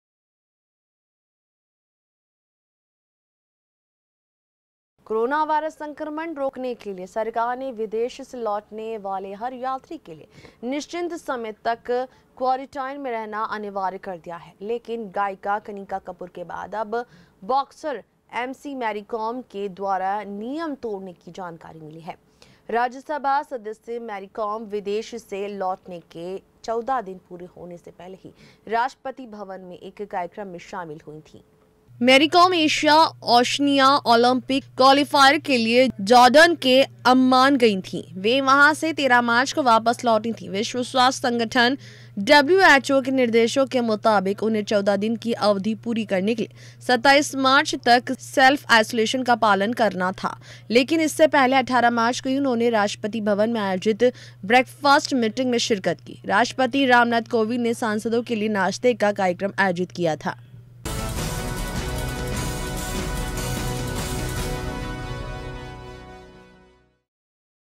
New Delhi -मैरी काॅम ने क्वारैंटाइन प्रोटोकाॅल तोड़ा
कोरोनावायरस संक्रमण रोकने के लिए सरकार ने विदेश से लौटने वाले हर यात्री के लिए निश्चित समय तक क्वारैंटाइन में रहना अनिवार्य कर दिया है। लेकिन गायिका कनिका कपूर के बाद अब बाॅक्सर एमसी मैरी काॅम के द्वारा नियम तोड़ने की जानकारी मिली है। राज्यसभा सदस्य मैरी काॅम विदेश से लौटने के 14 दिन पूरे होने से पहले ही राष्ट्रपति भवन में एक कार्यक्रम में शामिल हुई थीं।